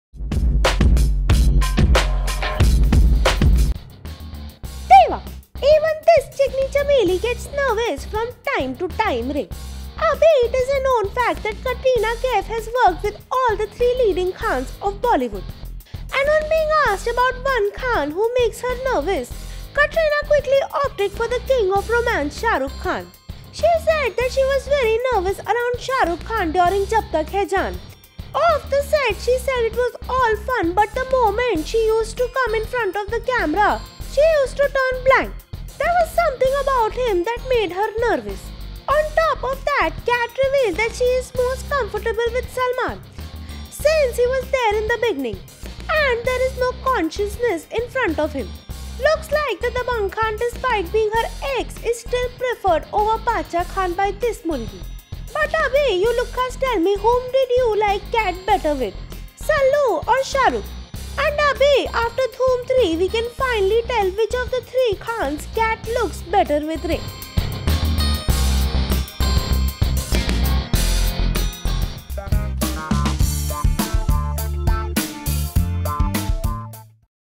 Katrina Kaif Says SRK Makes Me Nervous!
SRK makes me nervous, while Salman comfortable: Katrina- On being asked about one Khan, who makes her nervous, Katrina quickly opted for the King of romance, Shahrukh Khan. "I was very nervous around Shahrukh Khan during Jab Tak Hai Jaan. Off the sets, it was all fun, but the moment I used to come in front of camera, I used to turn blank. There was something about him that made me nervous," said Katrina.Kat revealed that she is the most comfortable with Salman. "I am very comfortable with Salman as he has seen me since the beginning and there's a comfort level. There is no consciousness in front of him," concluded Katrina.
Looks like the dabang khan despite being her ex is still preferred over the Badshah Khan by this mulgi.